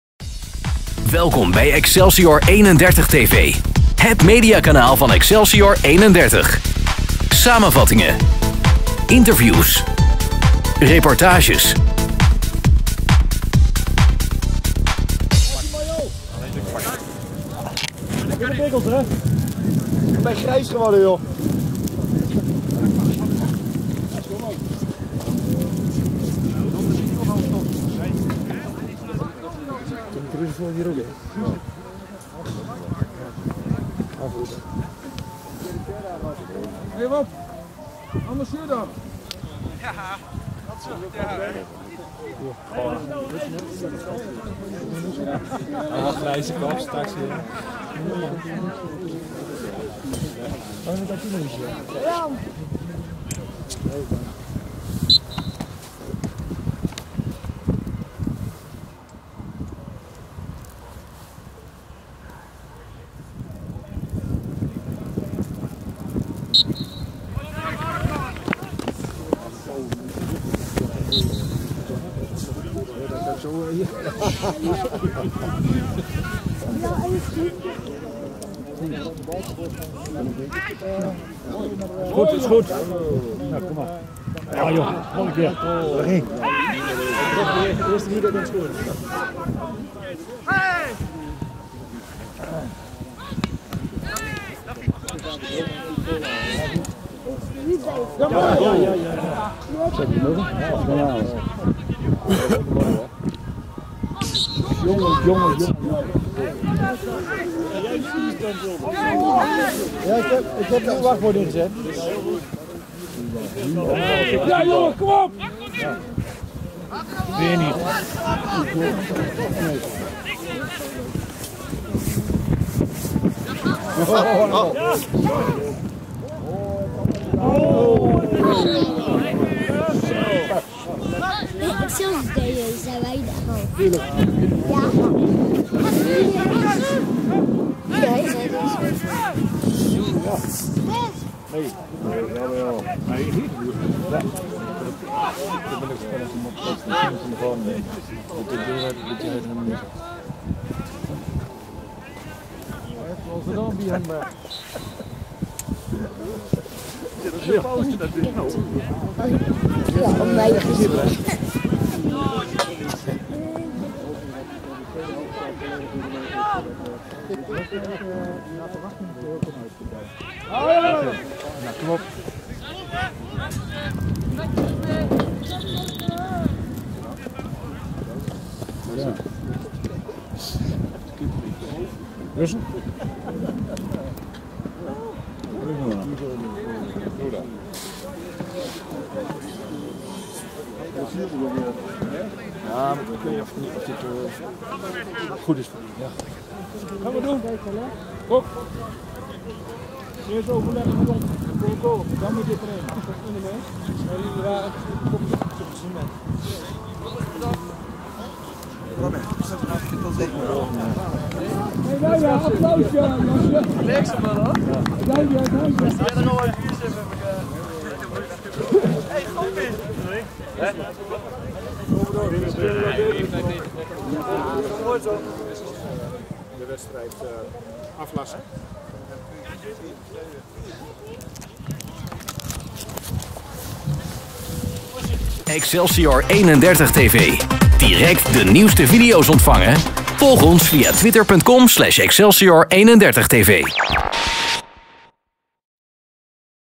DOS'37 - Excelsior'31
Het oefenduel tussen zaterdag 1e klasser DOS'37 en Excelsior'31 is na ca. 40 minuten spelen gestaakt vanwege de aanhoudende sneeuwval.

De veiligheid van de spelers kon hierdoor niet worden gewaarborgd, dus besloot de arbiter het duel definitief af te blazen. De stand op dat moment was, zoals het duel begon, 0-0.

Komende week speelt Excelsior'31 een cruciale thuiswedstrijd, tegen Jodan Boys. De achterstand op de Goudanaren bedraagt inmiddels 8 punten. Met een overwinning hebben de rood/witten Jodan Boys weer enigszins in het vizier.